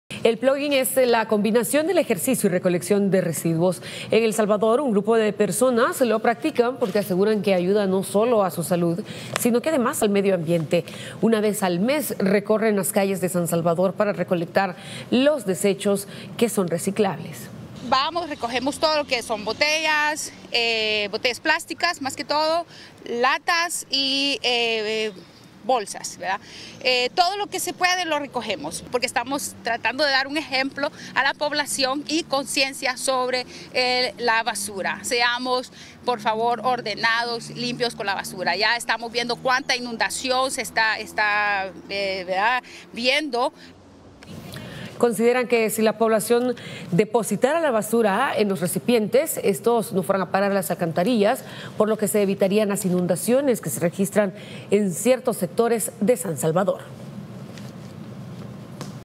Teleprensa33 | Una vez al mes 30 personas corren y recogen residuos
El plogging es la combinación del ejercicio y recolección de residuos.